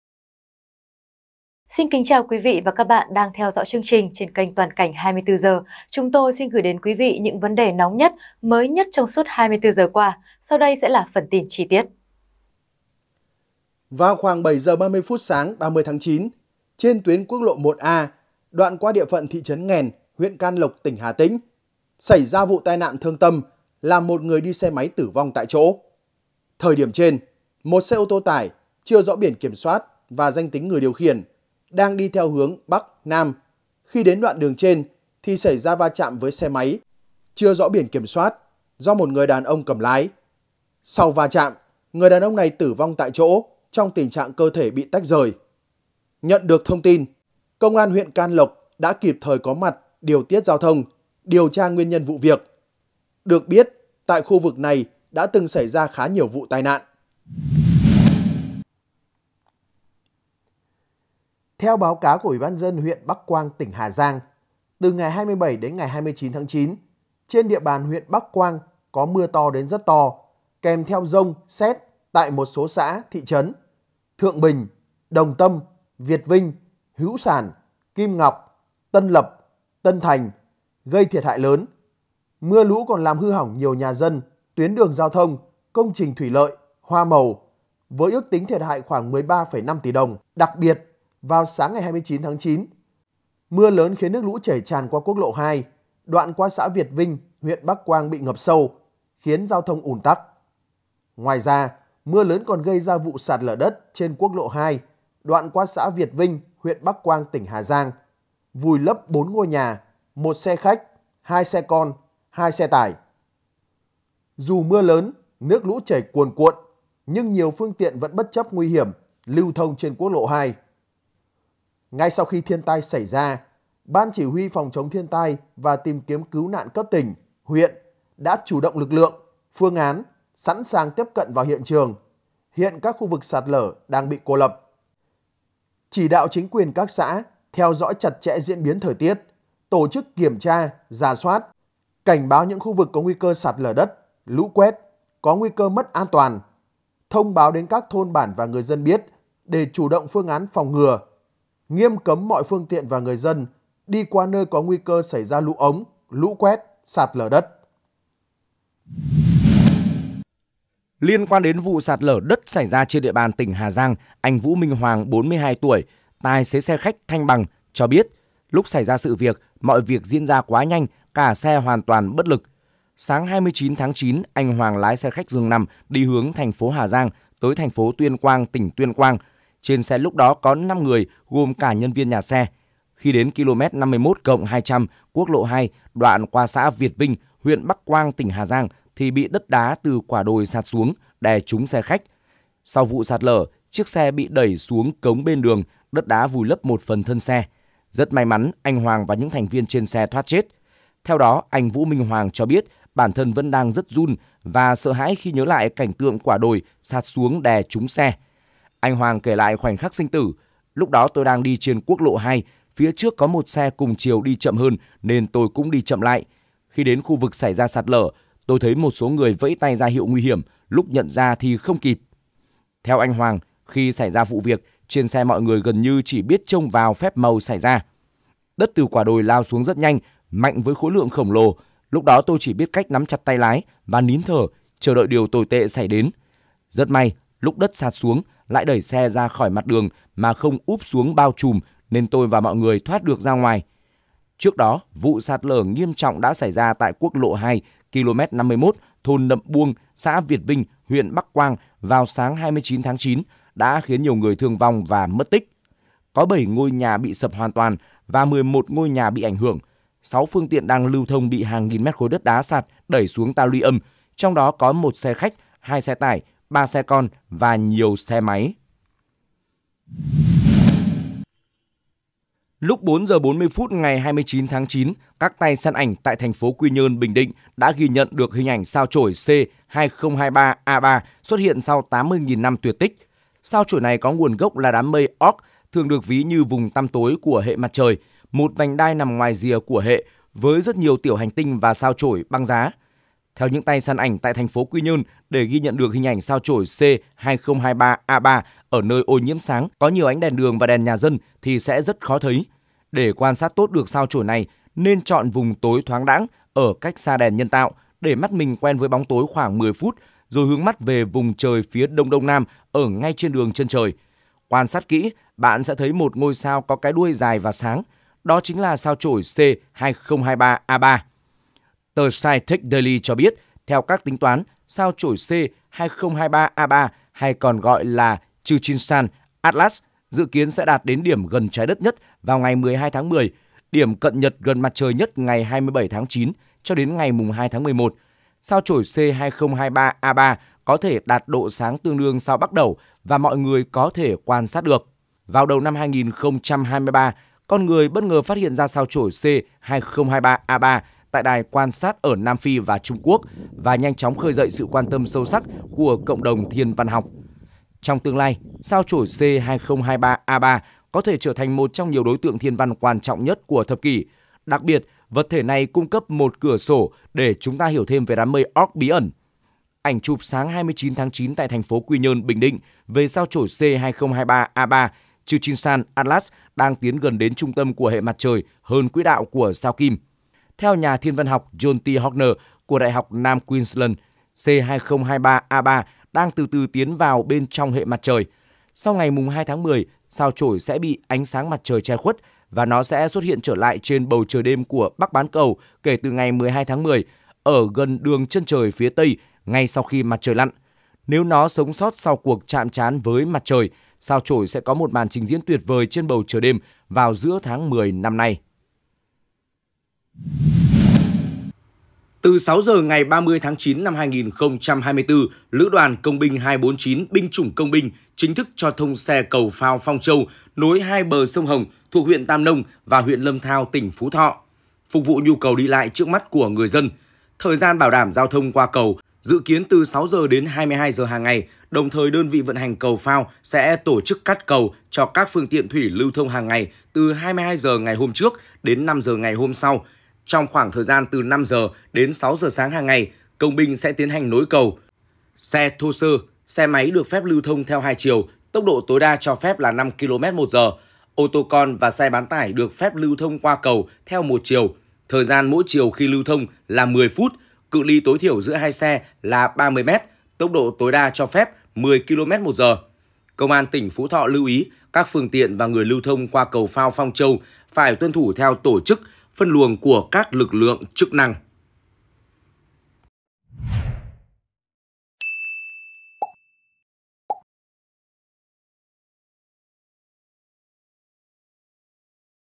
Một người tử vong thảm khốc sau va chạm với ô tô tải ở Hà Tĩnh
Toàn cảnh 24h | Một người tử vong thảm khốc sau va chạm với ô tô tải ở Hà Tĩnh
Vào khoảng 7h30 sáng 30/9, trên tuyến quốc lộ 1A đoạn qua địa phận thị trấn Nghèn (huyện Can Lộc, tỉnh Hà Tĩnh) xảy ra vụ tai nạn thương tâm làm một người đi xe máy tử vong tại chỗ.